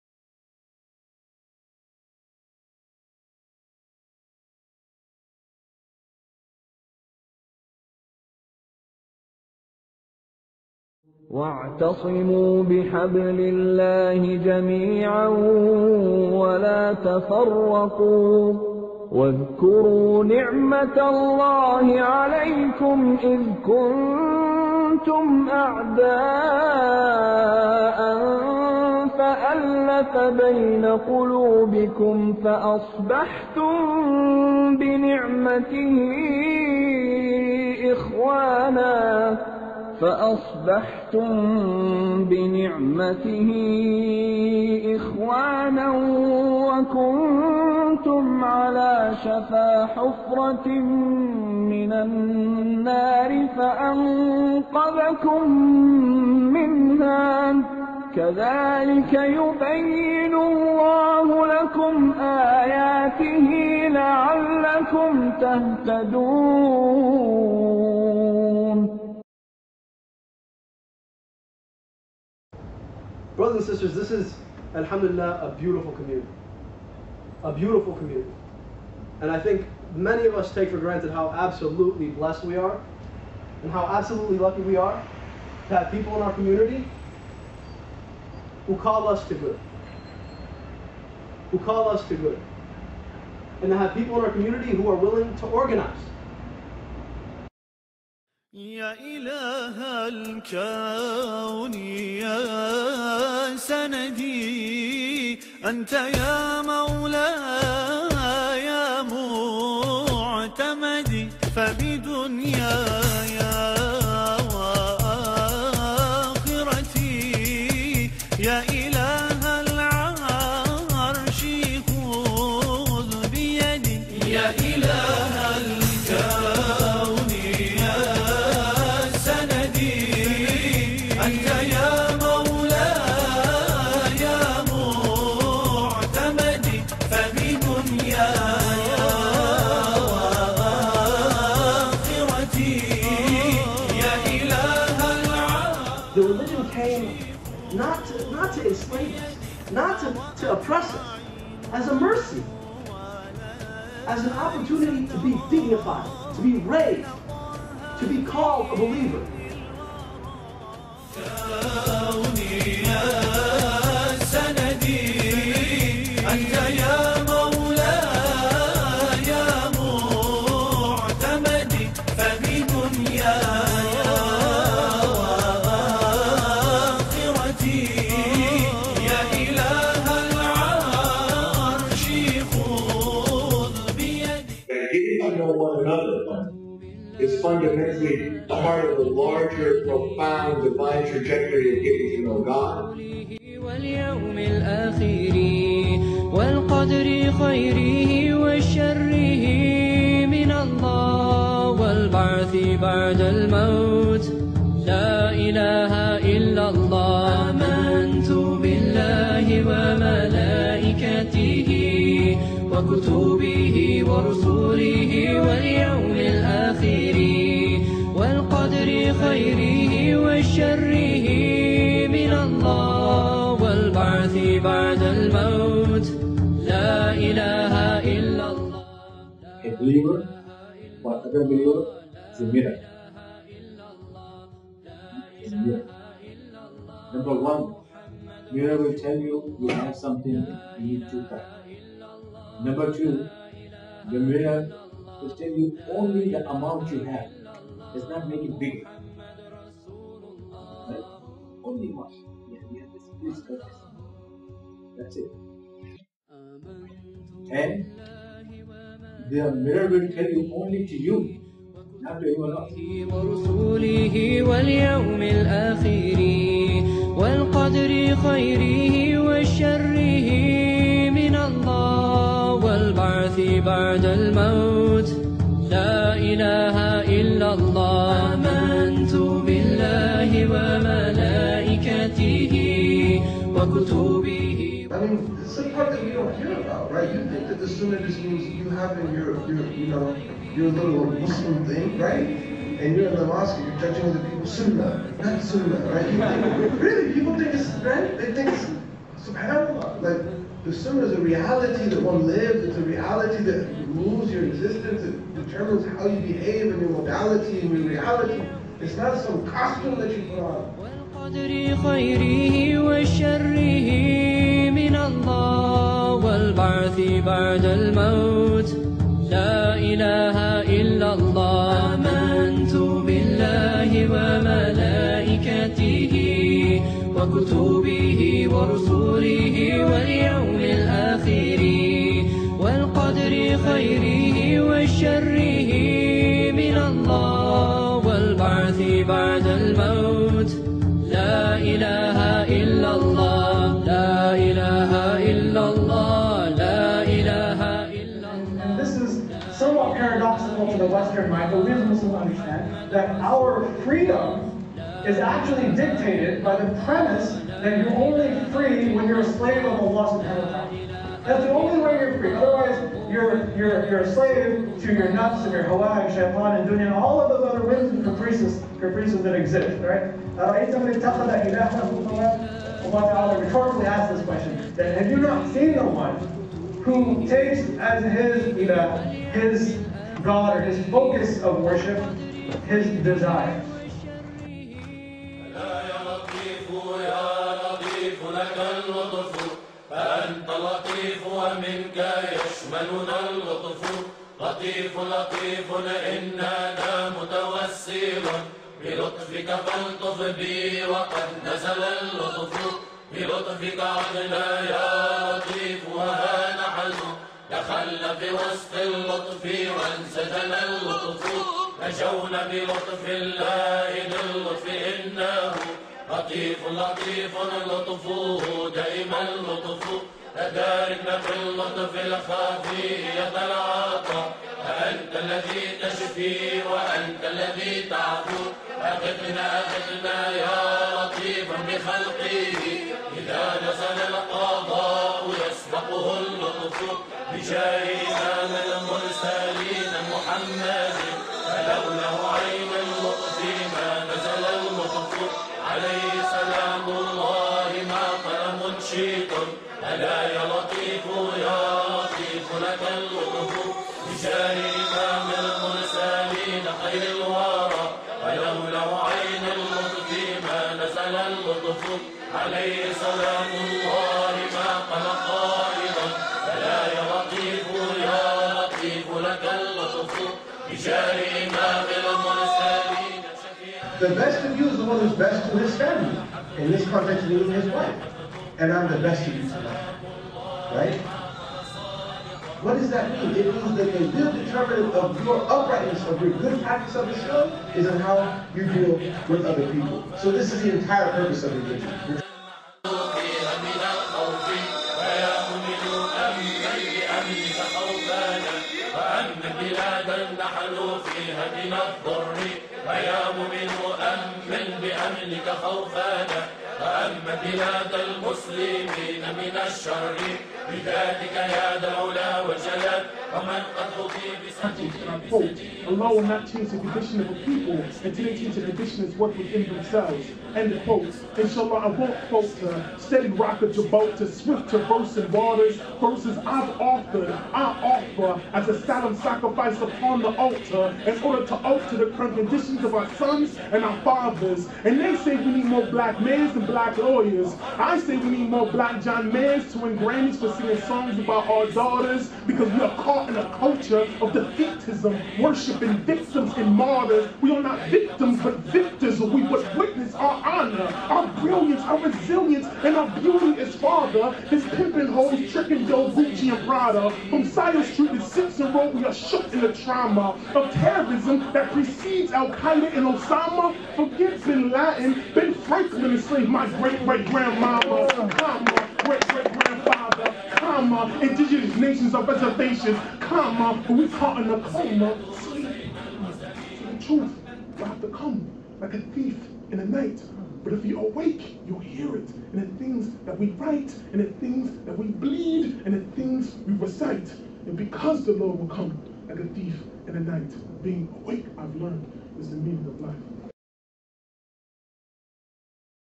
University of Michigan MSA Trailer 2009-2010
Trailer showcasing the activities of the Muslim Students Association at the University of Michigan-Ann Arbor. Premiered at the Michigan Muslims Banquet on February 12, 2010